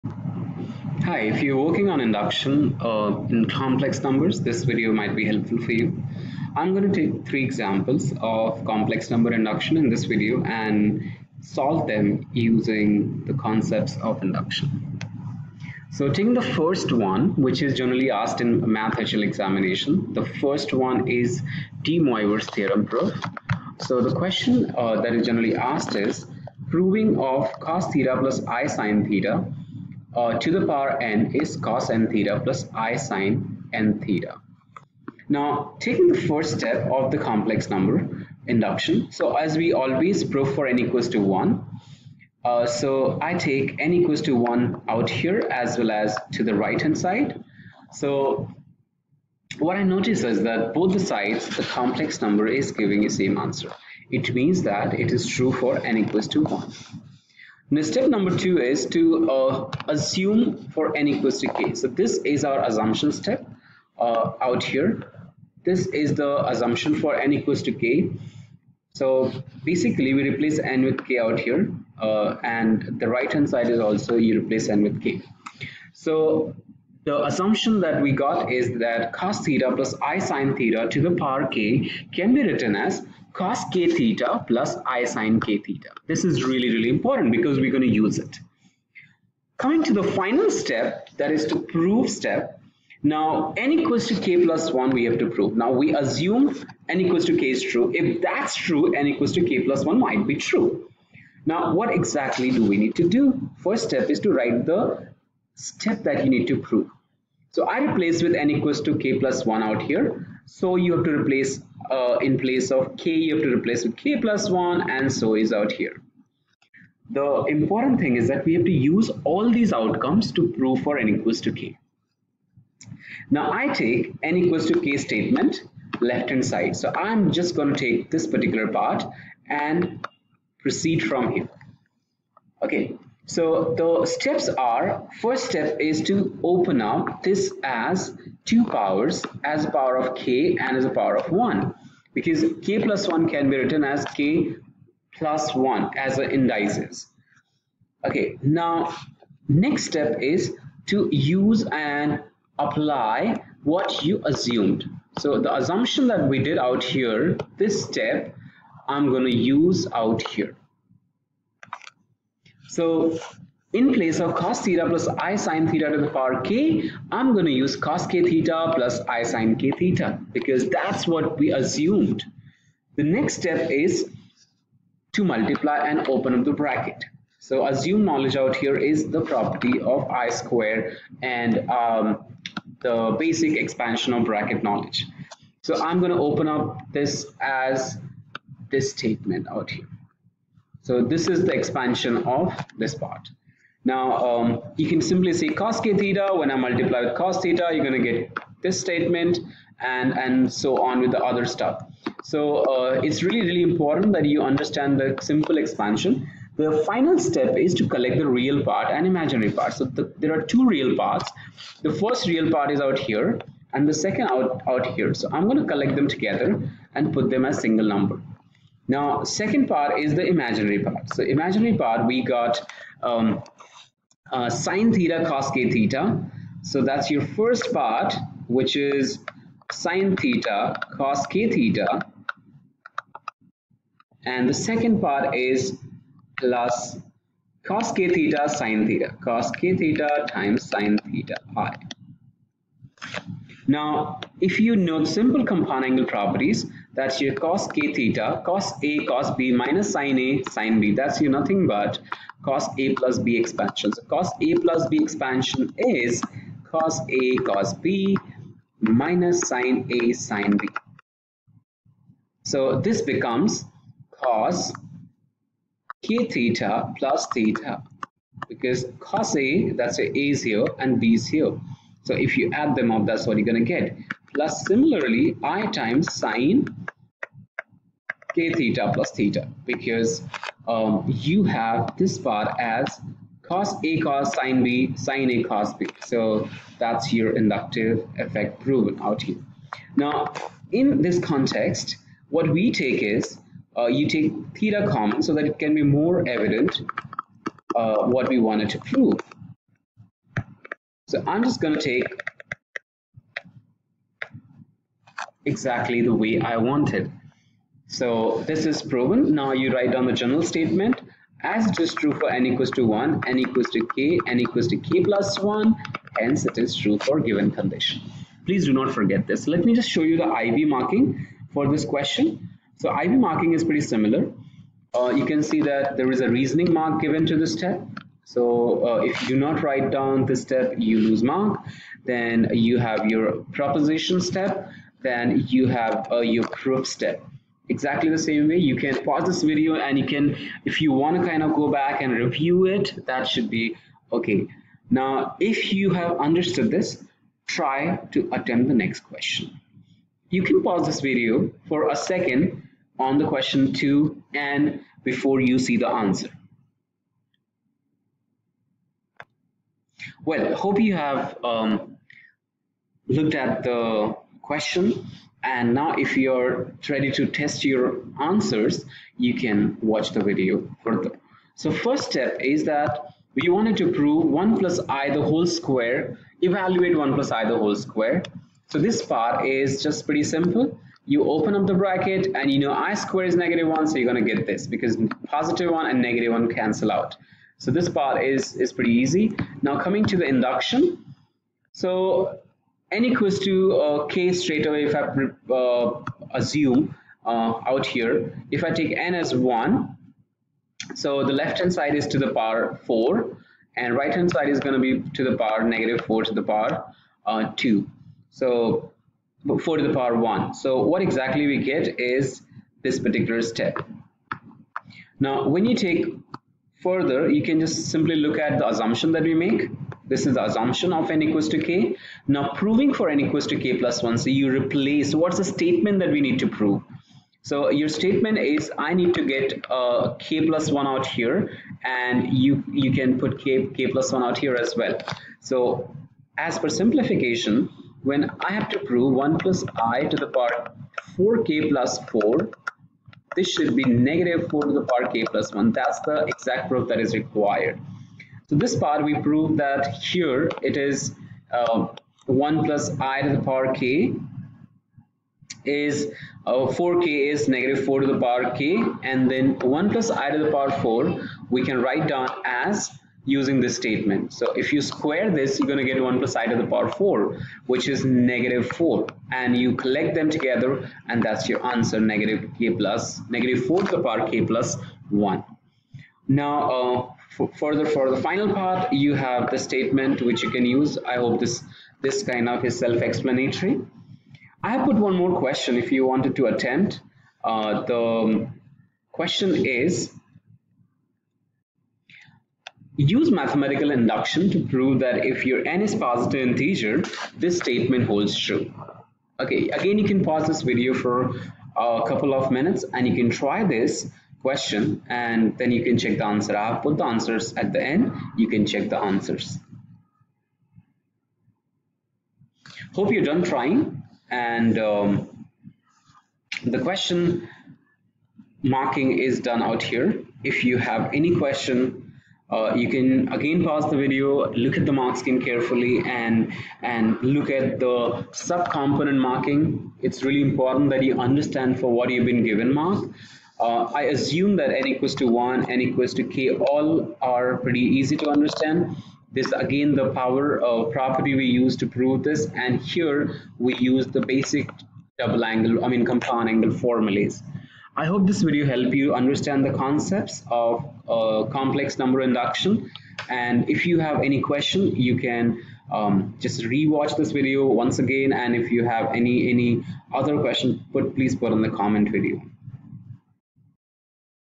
Induction and Complex number
This helps you understand concepts of Induction in complex numbers: IB Math HL exam Questions